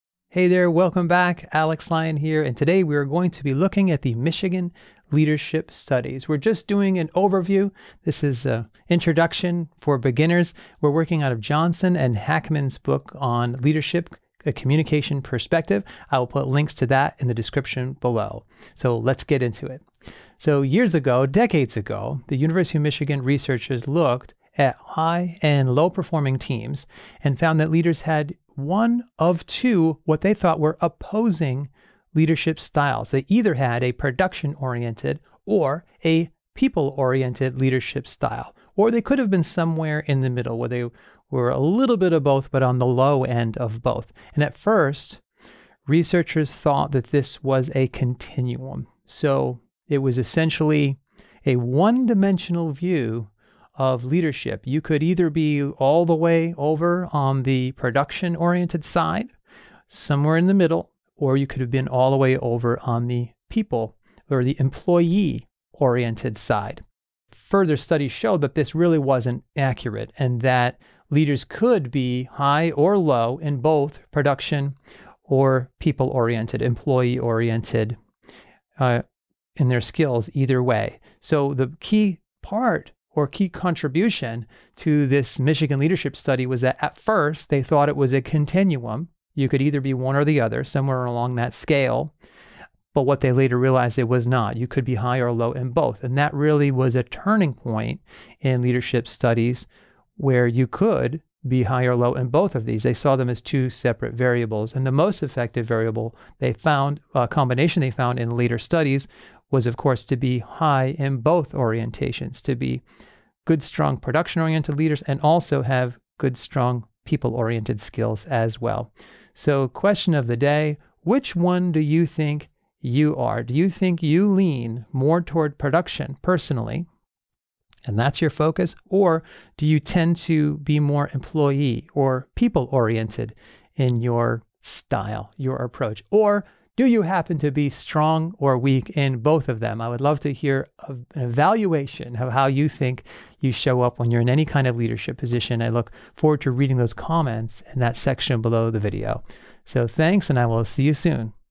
Michigan Leadership Studies
The Michigan Leadership Studies laid a foundation for the production-orientation vs people-orientation debate in the 1950s and beyond. These studies, like many that came after them, contribute to the overall area of task and interpersonal relationship behaviors of leaders.

The Organizational Communication Channel provides supplemental lessons for traditional, hybrid, and 100% online courses for students and teachers everywhere.